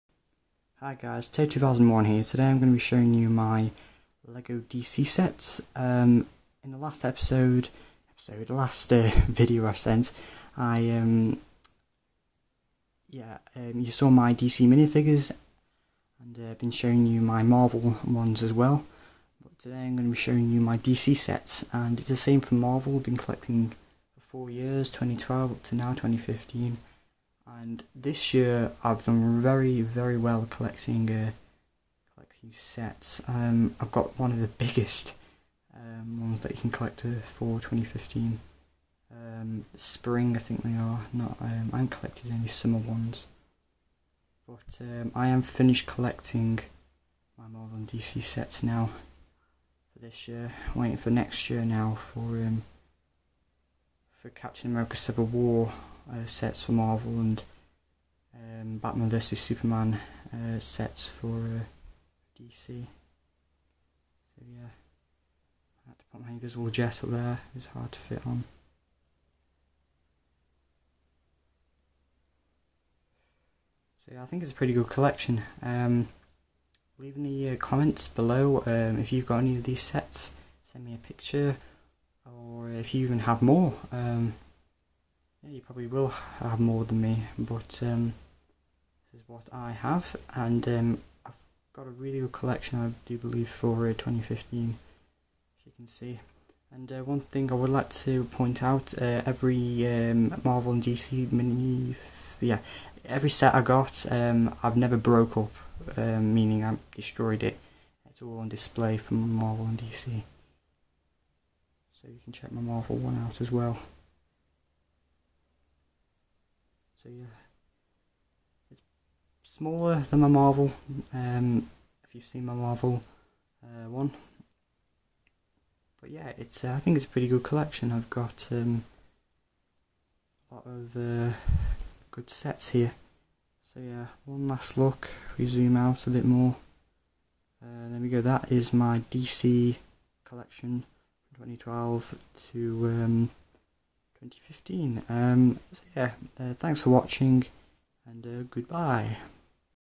LEGO: DC | My Sets Collection (2012 - 2015)
I'm today showing you my Lego DC set collection enjoy!!! And thanks for the 10 sub's!!!